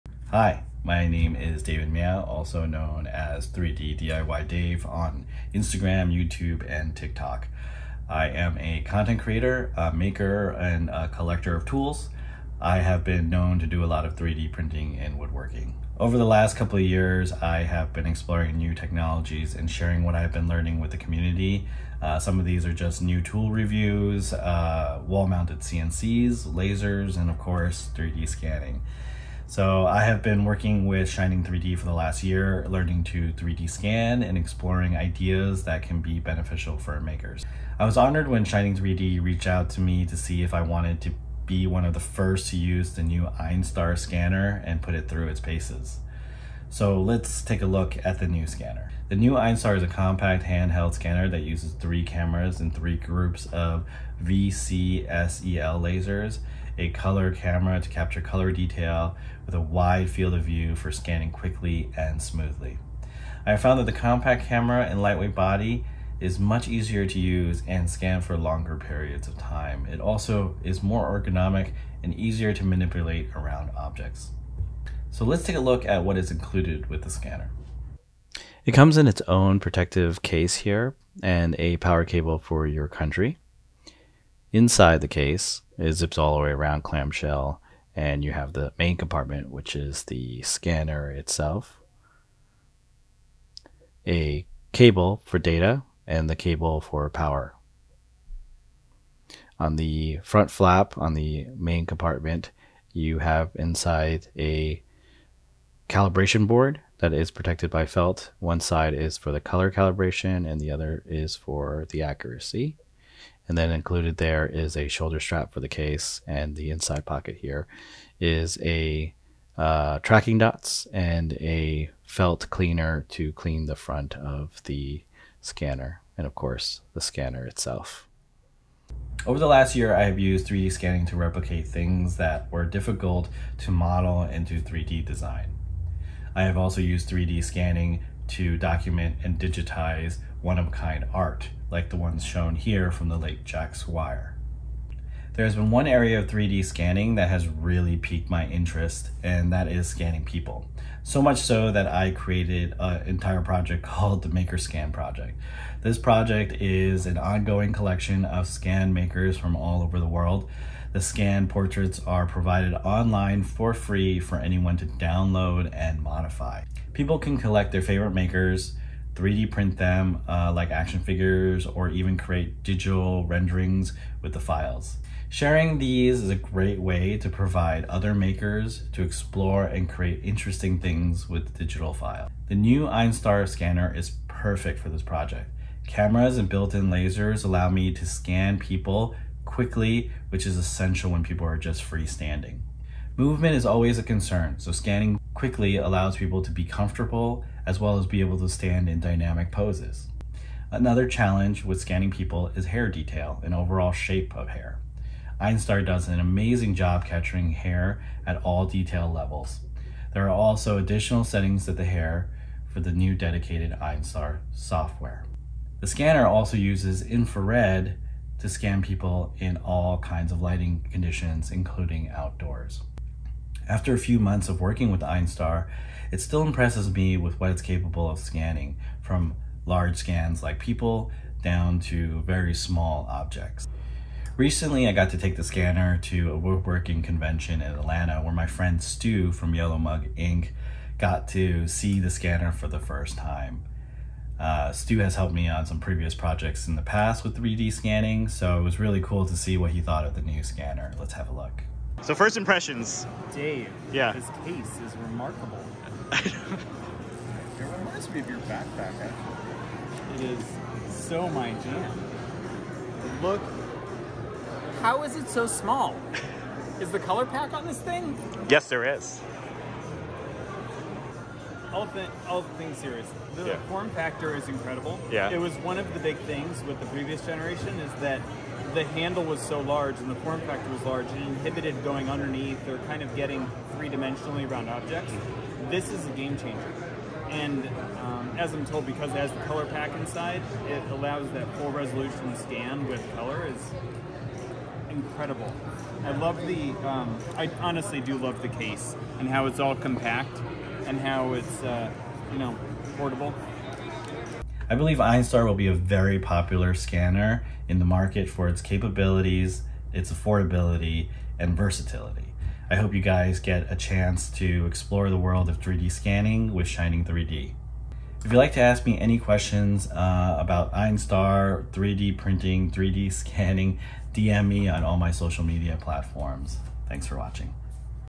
Digitizing People for an Open-Source Project with Einstar
I have had the pleasure to test and use Shining 3D new Einstar 3D scanner the last few months. This scanner will be a great first scanner for people just getting into 3D scanning. From scanning small detailed models to people. Einstar makes easy work of it all.